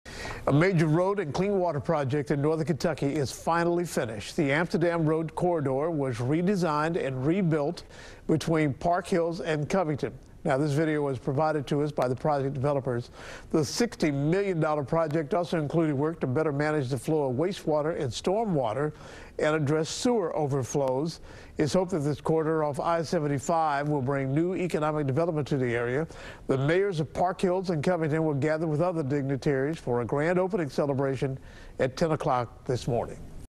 ❤️ Amsterdam Road Corridor by Joshua One💚WKRC-TV Local 12 CBS Cincinnati💜 2020 PR by Andy Hemmer 💜
An enormous leap forward by the Clean H2040 program in Kentucky was recently celebrated on the Covington/Park Hills city line, hosted by Joshua One, “A Developer with a Different View,” and its project partners.

What is Clean H2O40?

Clean H2O40 is SD1’s commitment to “increasing capacity to better manage the flow of wastewater and storm water in Northern Kentucky.”

SD1 has teamed up with the Environmental Protection Agency (EPA) and the Commonwealth of Kentucky to balance the need to keep local waterways clean and safe, with the need to provide affordable wastewater and storm water services in Northern Kentucky.

Through a number of strategic projects specifically designed to address sewer overflows, SD1 will meet the requirements of Northern Kentucky’s amended consent decree and improve the quality of life.

By the year 2040, Clean H2040 plans to “completely eliminate typical-year sanitary sewer overflows and recapture at least 85 percent of all typical-year combined sewer overflows.”

AMSTERDAM ROAD REGIONAL RETENTION BASIN courtesy

The shareholders in the Amsterdam Road Regional Retention Basin partnership have invested millions of dollars, and years of sweat and service, comprising an innovative team of leading government, public agency and private development officials and their staff from:

- Kenton County, led by Judge Executive Kris Knochelmann;
- The City of Park Hills, led by Mayor Kathy Zembrodt;
- The City of Covington, led by Mayor Joseph U. Meyer;
- Sanitation District №1 (SD1), the local utility managing Northern Kentucky’s wastewater and storm water, led by Executive Director Adam Chaney;
- Planning & Development Services of Kenton County, led by Director of Planning and Zoning Emi Randall;
Joshua One, developer of The Views on Dixie Highway and developer of the upcoming Park Pointe in Covington/Park Hills, a major catalyst of the infrastructure improvements. JoshuaWon is represented by developer Paul Zeltwanger and Director of Construction Bryan Bailin.

Joshua One recently expanded and relocated to the new Joshua One Design Center on East Eighth Street in Newport, as recently featured in The Cincinnati Business Courier:

Thx again to WKRC-TV Local 12 for covering the Amsterdam Road Corridor partner celebration and official shout-out.
# 30 #